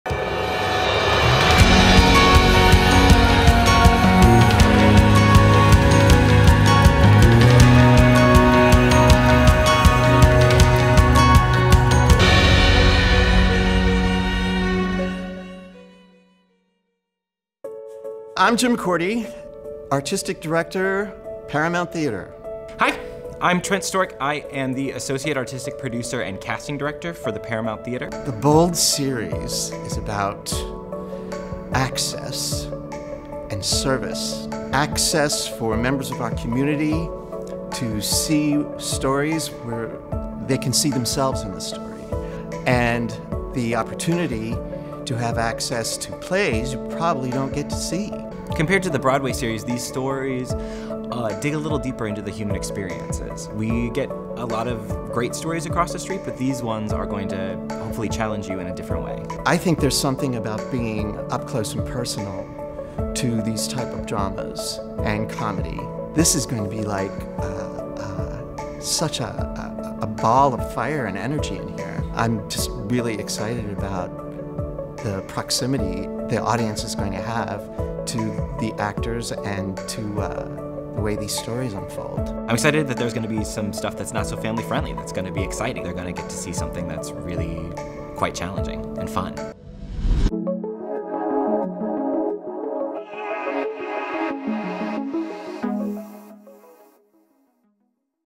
A BOLD Conversation
Go inside what the BOLD Series in with Artistic Director Jim Corti and Associate Artistic Director Trent Stork.